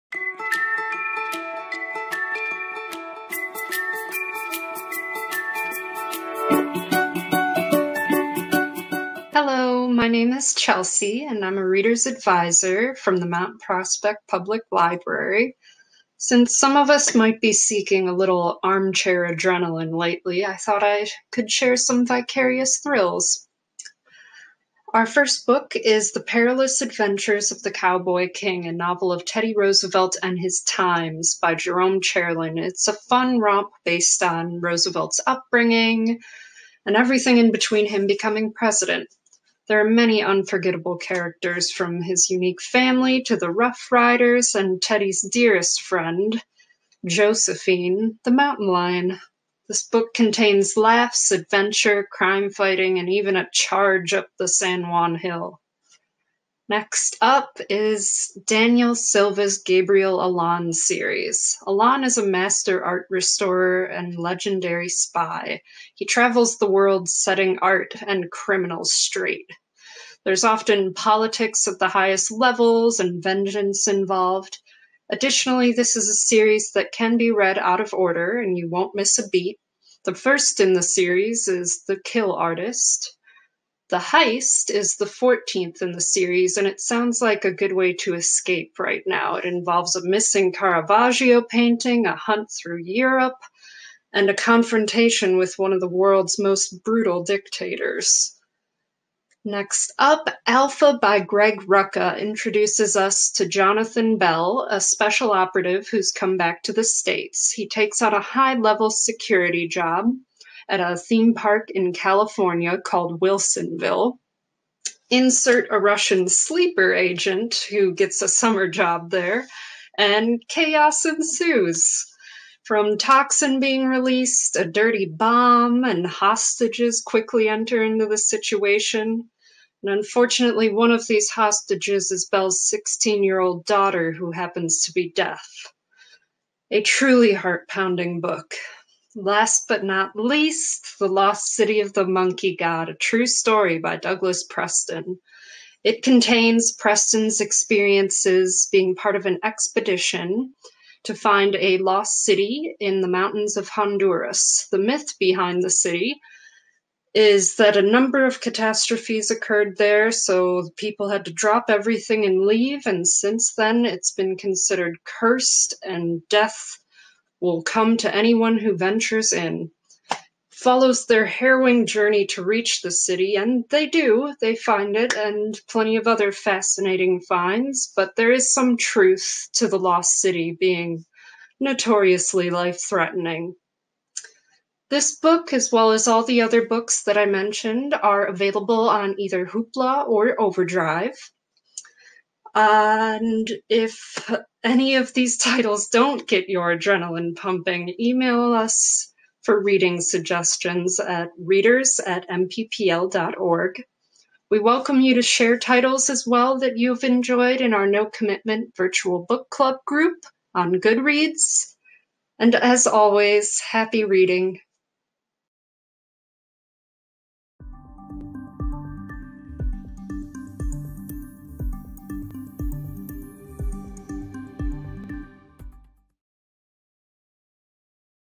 Booktalk: Vicarious Thrills
Readers' Advisor Chelsea brings us several suggestions on books to read for vicarious thrills; including a book featuring a president with a pet mountain lion and a thrilling ongoing series with an art restorer turned spy.

The Perilous Adventures of the Cowboy King: a Novel of Teddy

Daniel Silva's Gabriel Allon series (1st in series The Kill Artist on Overdrive series where you may go out of order (a number of titles are avail. on Hoopla as well) --The Heist (14th in series) avail. on eAudio on Hoopla and both eBook and eAudio on Overdrive

Alpha by Greg Rucka (eAudio -Overdrive)

The Lost City of the Monkey God by Douglas Preston (eAudio-Hoopla eAudio+eBook -Overdrive)